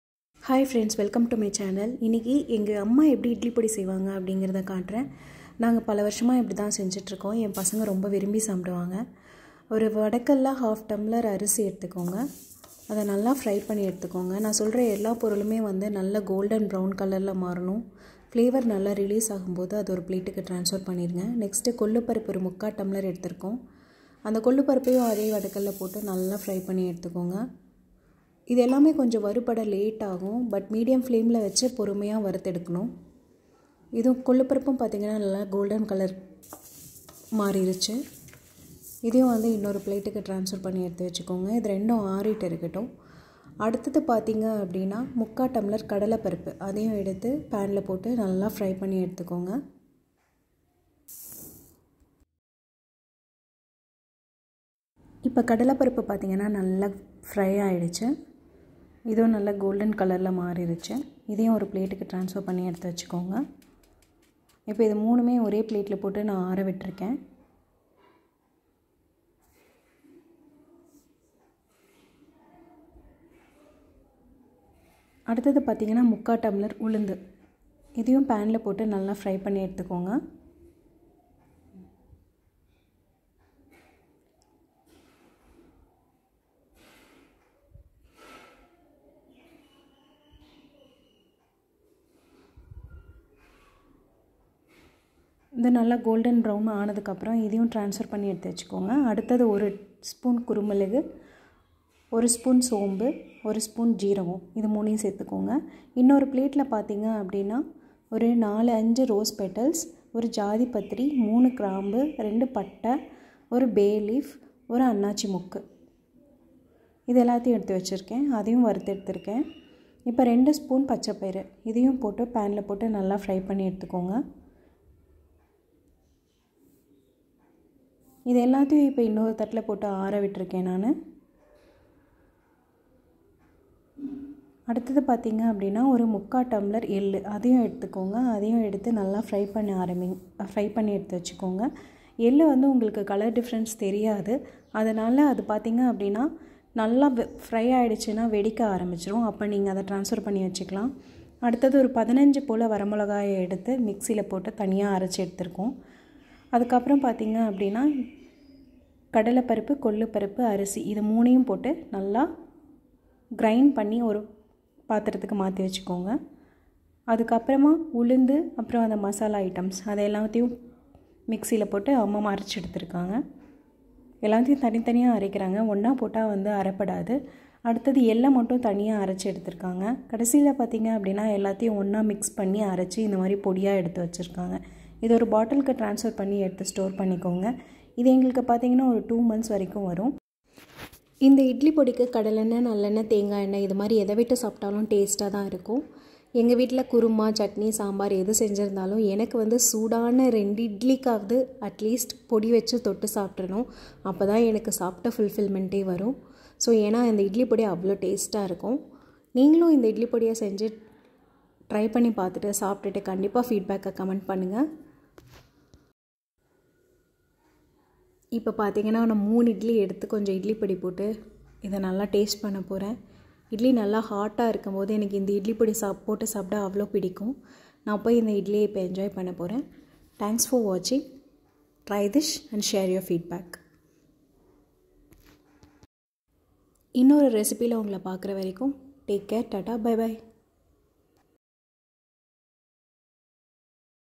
அம்மா ஸ்டைல் இட்லி பொடி | Idly podi | different method of healthy idly podi by amma
Ingrediants

Any Rice (ponni rice) - 1/2 tumler
channa dal - 3/4 tumler
urad dal -3/4 tumler
sesame seeds black -3/4 tumler
horse gram  -3/4 tumler
dry red chilli - 15 to 20 based on it's hot
green gram  - 2 tablespoon
black pepper - 1 tablespoon
cumin- 1 tablespoon
fennel seeds - 1 tablespoon
rose petals optional - 5 leaves
bay leaf - very little
cloves - 3
mace- small piece
cinnamon - small one
Marathi moggu (kapok buds) - 1 small
star anise- half